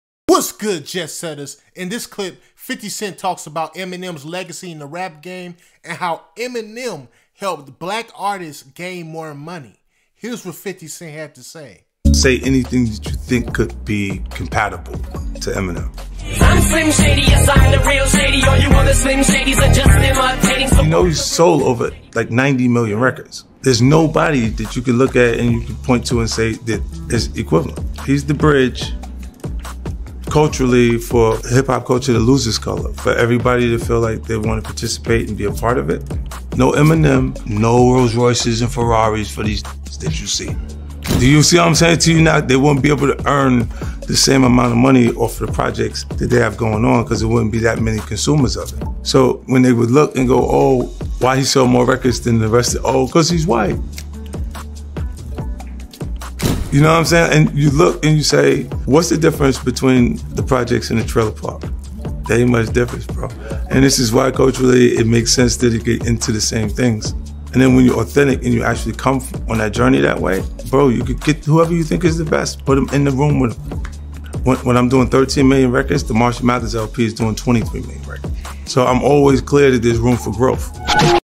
50 Cent Explains How Eminem Helped Black Artists Make More Money
50 Cent gives his reasoning as to why he think Eminem helped black artists make more money.                                                                                                                              Signup For Fiverr